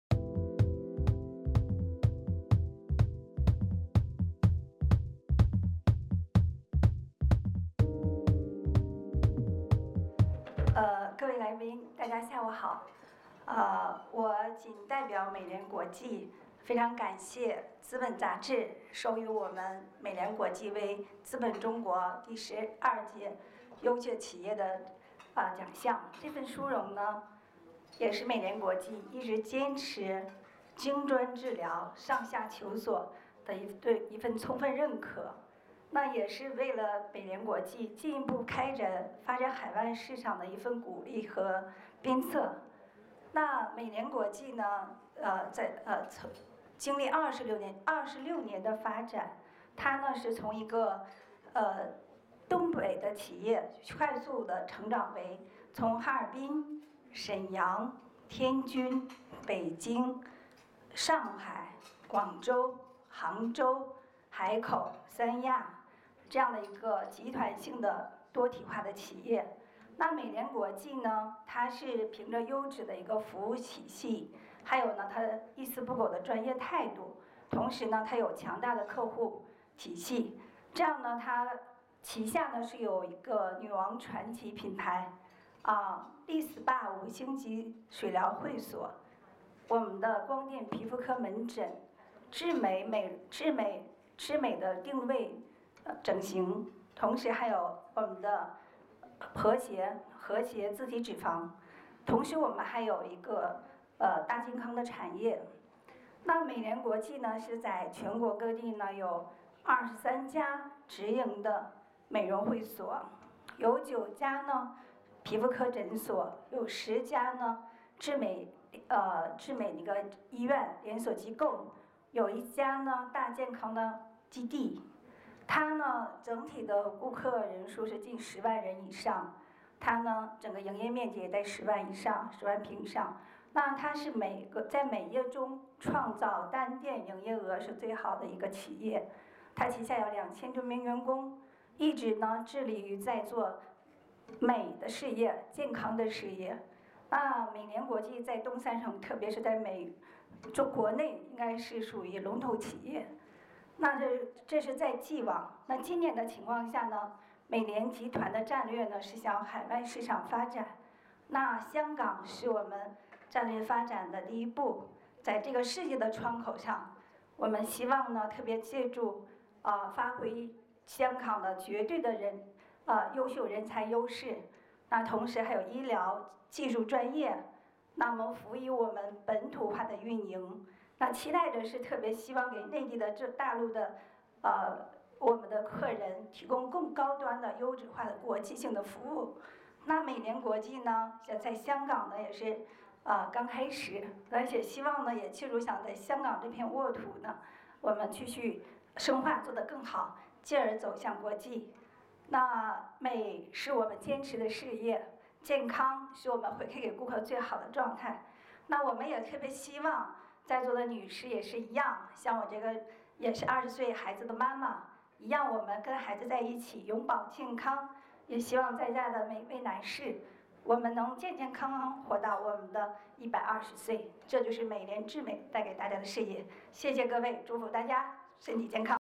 資本傑出中國醫學美容集團 - 美聯國際集團
《資本雜誌》「第十二屆資本傑出中國企業成就獎」 - 資本傑出中國醫學美容集團
得獎機構: 美聯國際集團
領獎代表：美聯國際集團醫學美容 總經理李雪飛女士